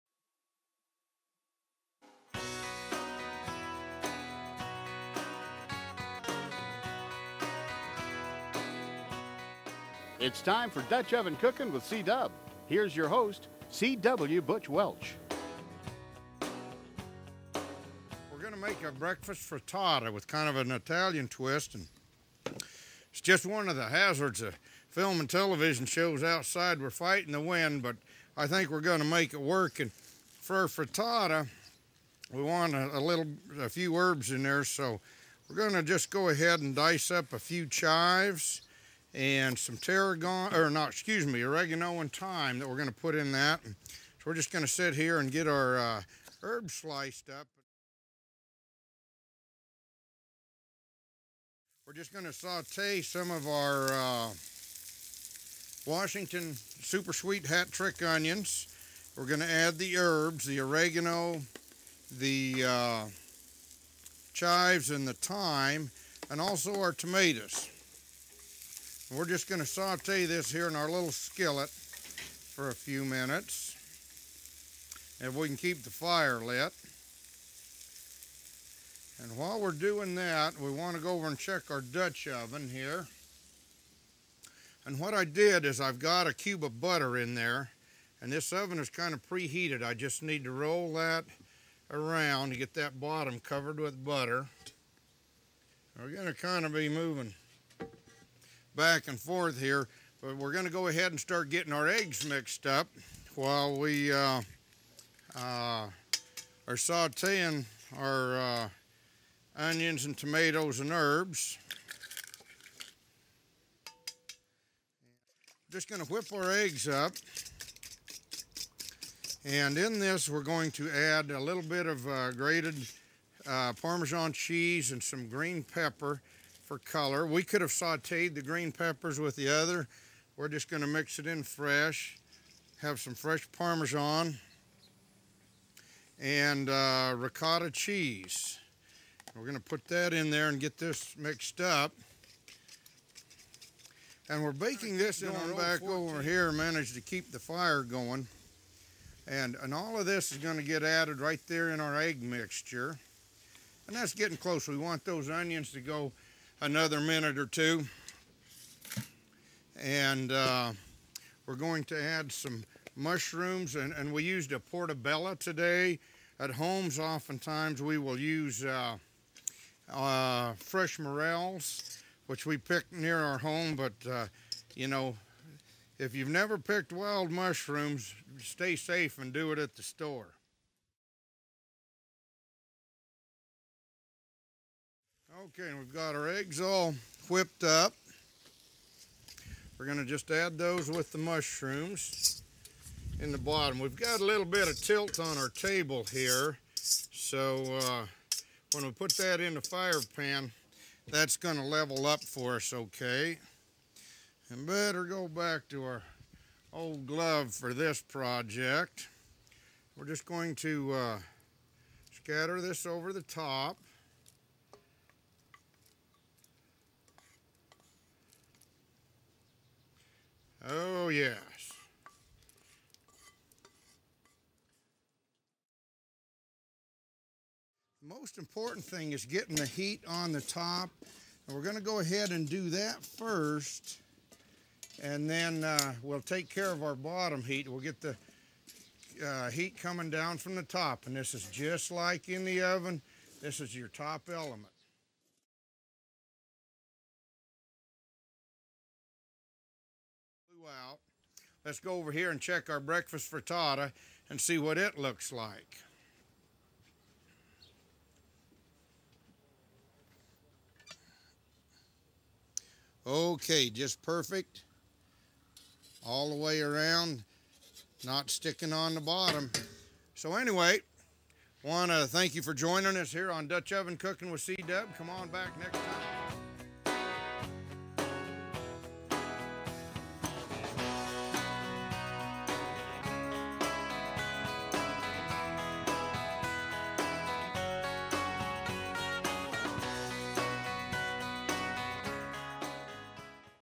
Dutch Oven Italian Breakfast Frittata | Cee Dub
Look up "Breakfast Recipes" in Dutch oven cook books and you're sure to find a "Mountain Man Breakfast" with potatoes, bacon, eggs with lots of cheese. But...in this recipe Cee Dub shows you how to make a very tasty breakfast fritatta spiced with fresh herbs to lighten the calorie count often associated with Dutch oven breakfast recipes!